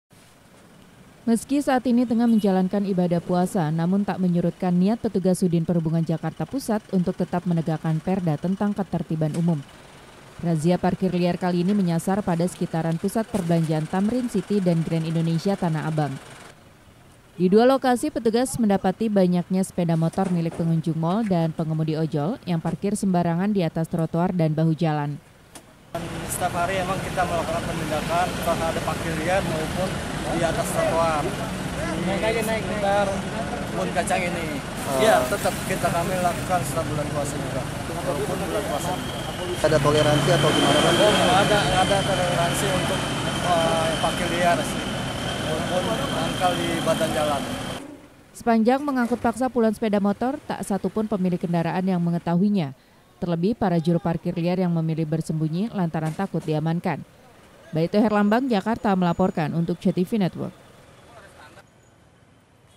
Puluhan Motor Parkir Sembarangan Tetap Diangkut Paksa
Masih nekat parkir sembarangan diatas trotoar dan bahu jalan, puluhan motor pengunjung dan ojeg online di Kawasan Pusat Perbelanjaan Thamrin City dan Grand Indonesia, Tanah Abang, Jakarta Pusat diangkut paksa petugas Sudin Perhubungan. Tak satupun pemilik sadar jika kendaraan mereka telah diangkut, sementara para juru parkir liar memilih bersembunyi.